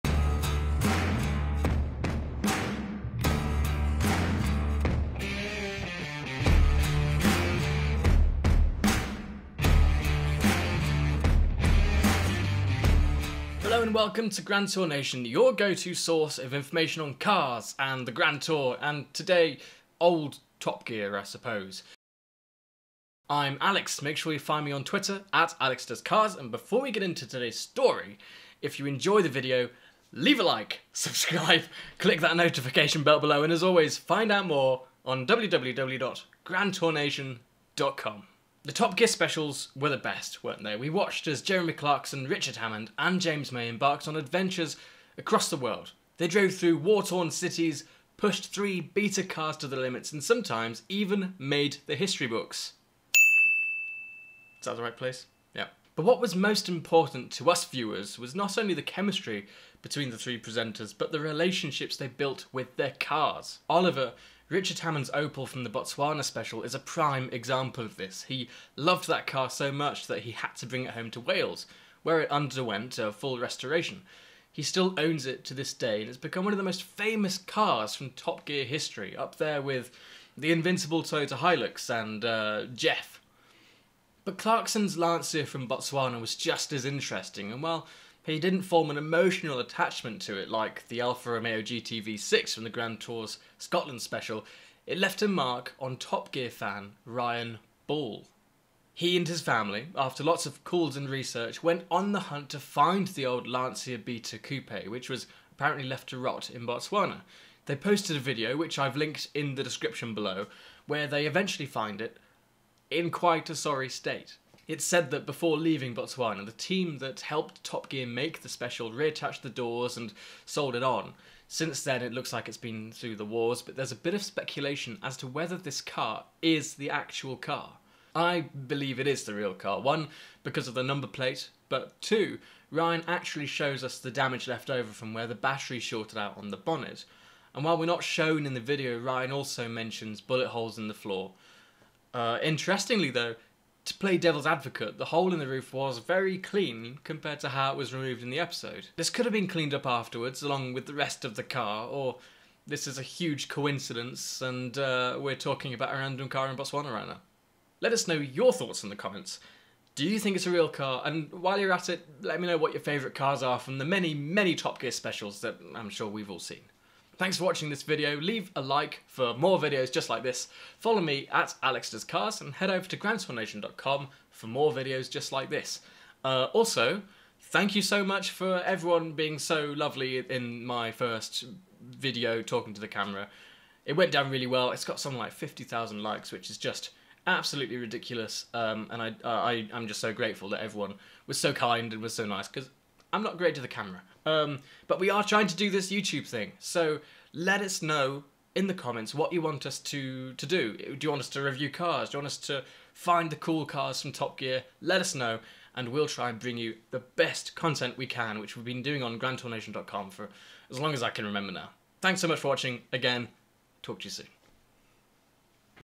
Jeremy Clarkson's Lancia Beta From Top Gear Special Has Been Found!
Jeremy Clarkson's Lancia Beta Coupe has been found in Botswana from the Top Gear Botswana Special.